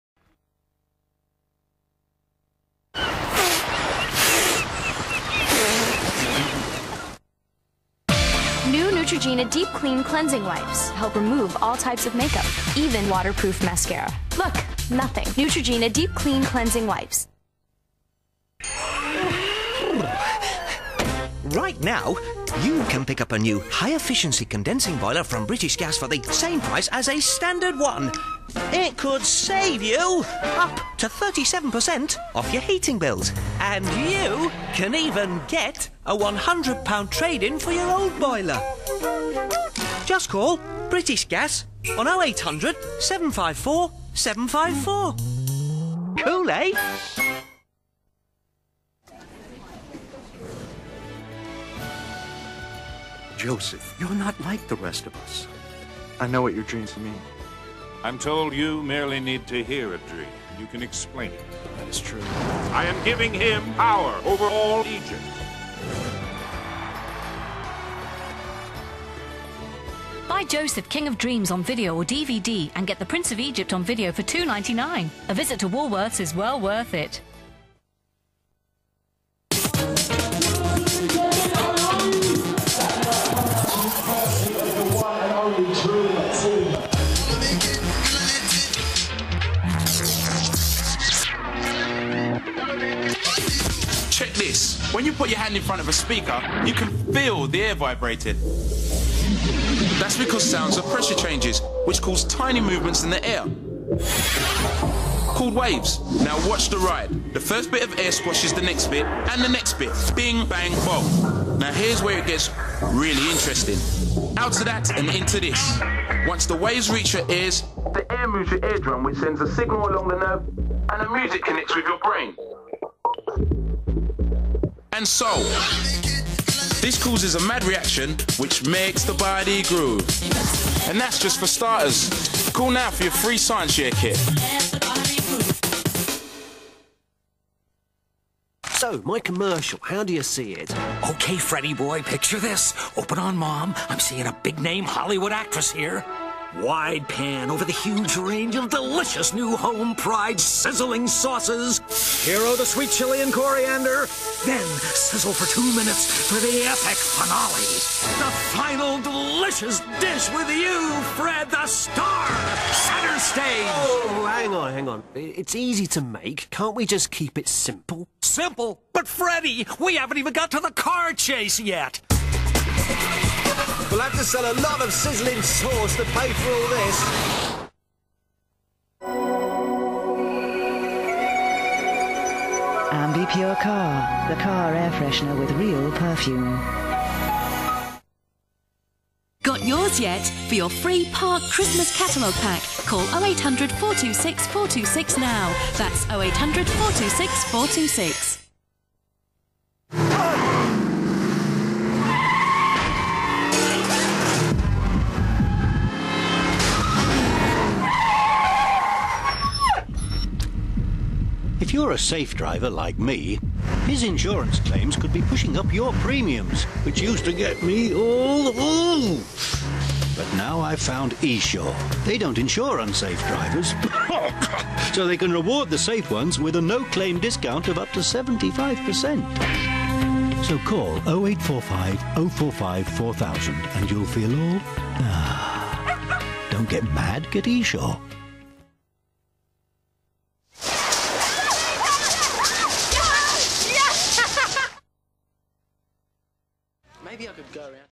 Channel 5 Ads 2001 (2)
Another Channel 5 advert break from October 2001, this one is complete.